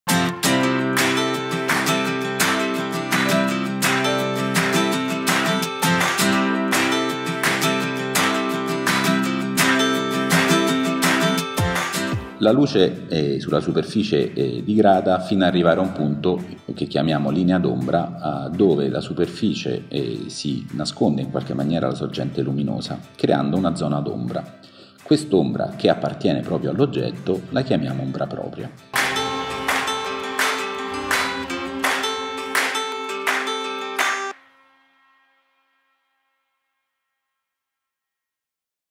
Teaser: minitutorial "Teoria delle Ombre"
Teaser: mini-tutorial "Cenni di Teoria delle Ombre" di Simone Gabrielli, disegnatore per Bonelli e insegnante della Scuola Romana dei Fumetti.